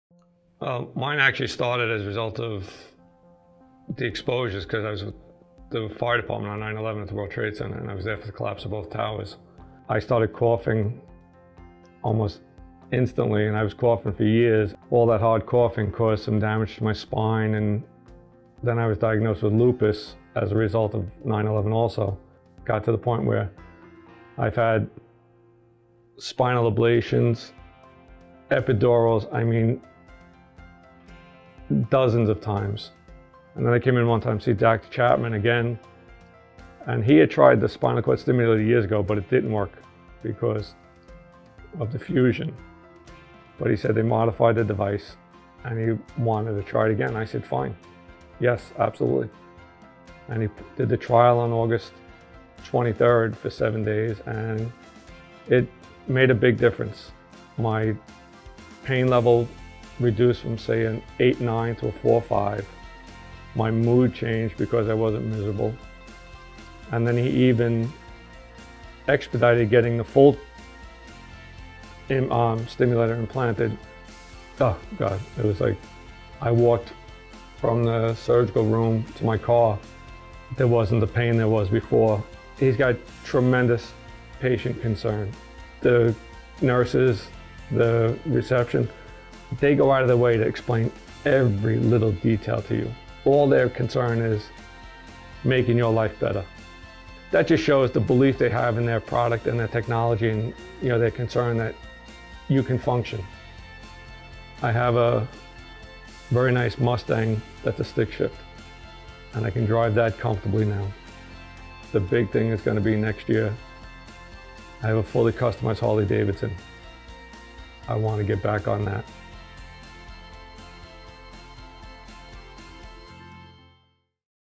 DRG Spinal Cord Stimulator at the T12 level for low back pain
Stephen was a first responder on 9/11, and as a result of exposure, his life - along with the nation’s - changed forever. His back pain was unbearable until Spinal Cord Stimulation returned his freedom.  He was incapacitate from pain and had a spinal fusion from T8 to his sacrum and had terrible pain, which also led to depression and anxiety, all of which are now greatly improved with this new treatment.

DRG SCS at the T12 level is an off label use for the dorsal root ganglion stimulator which is proving to provide better pain relief,  greater improvement in functionality/disability, and psychological status. Ask your physician about this new treatment.